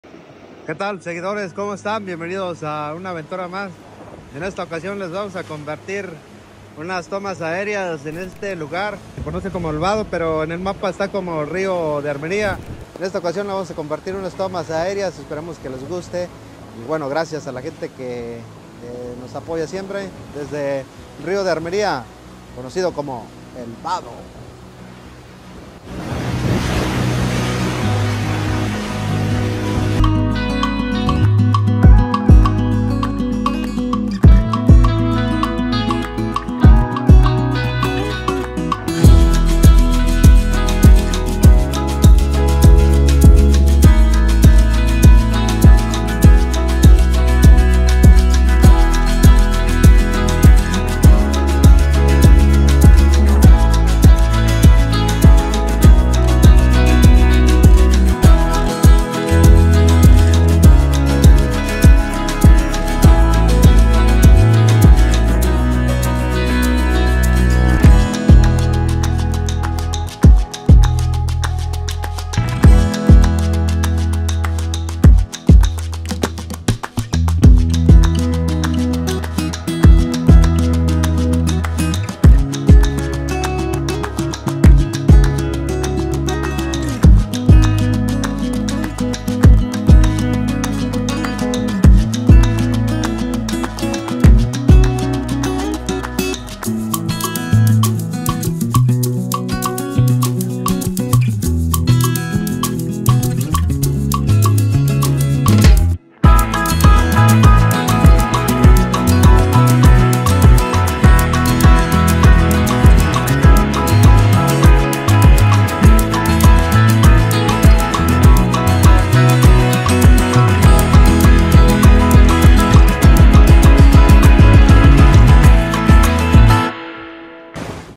Desde el vado unos de los mejores lugares donde vamos a nadar, ubicado en jalisco
síganme porque se vienes mas videos para el canal